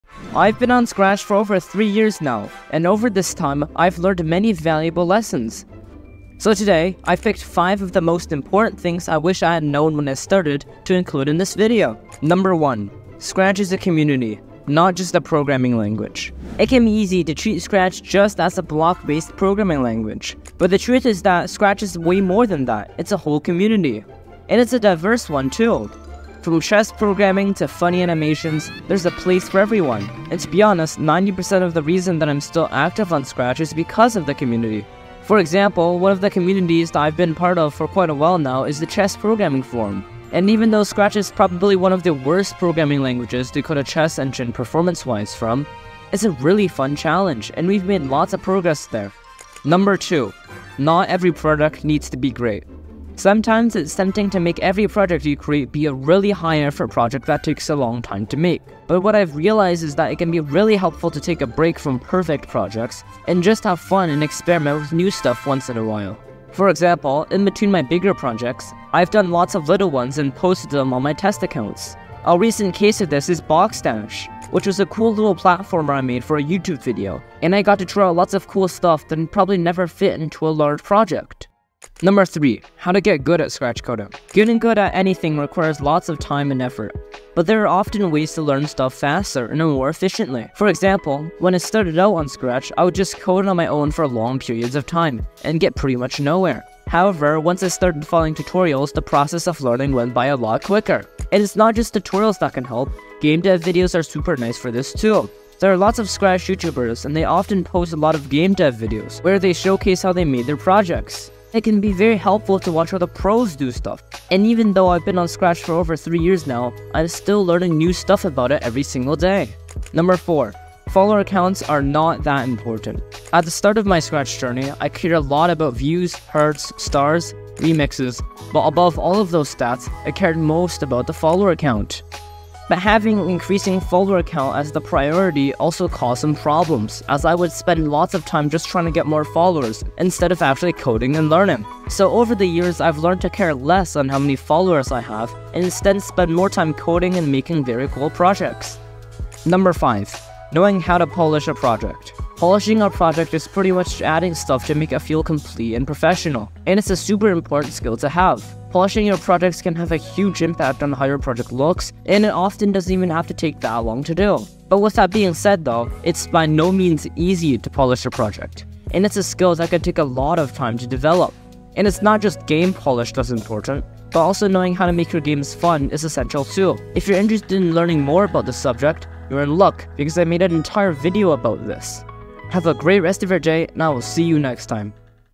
What I've Learned After Over 3 YEARS on SCRATCH!
I've been on Scratch for over 3 years (nearly 4) now! And in this time, I've learned lots of valuable lessons about Scratch! So today, I've collected 5 of the most important ones to put in this video!